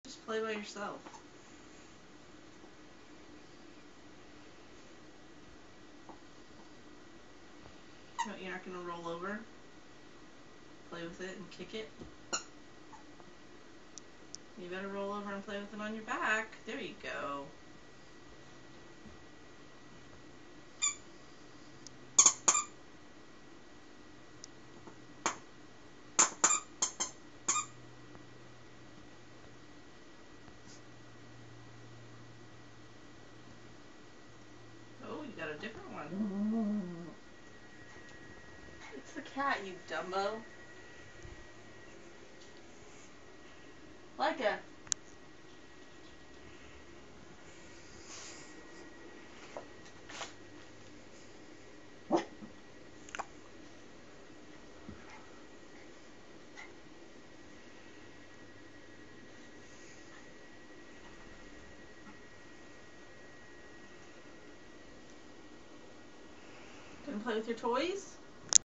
Laika plays with her toy by herself
Laika makes her own fun. Here she is playing with her own toy for a bit.